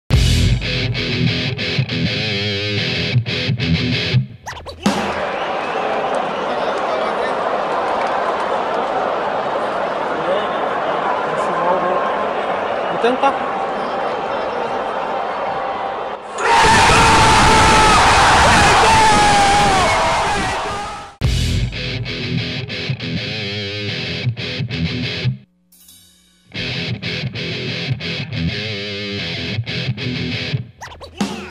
[DESCONTROLE] Grêmio 2 x 1 Atlético-MG (Gabriel Cirne) l GrêmioTV
O torcedor Gabriel Cirne gravou o gol do Luan contra o Atlético-MG das cadeiras superiores. Golaço e descontrole na Arena!